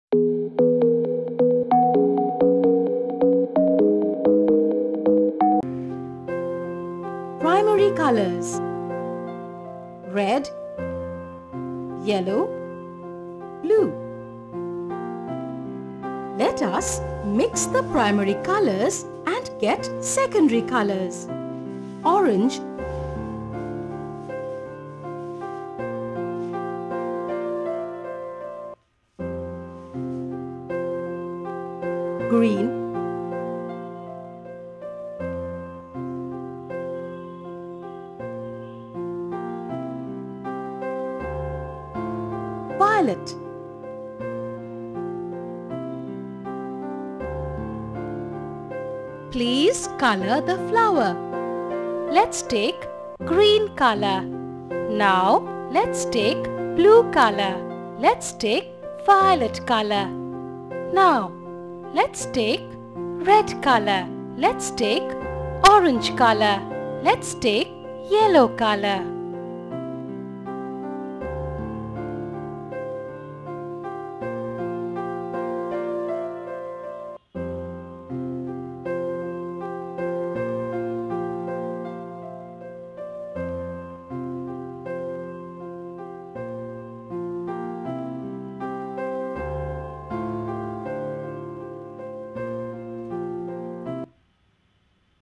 Lets Learn To Draw Colours || Colouring
This video we will see how to do color and the basic things are required for coloring.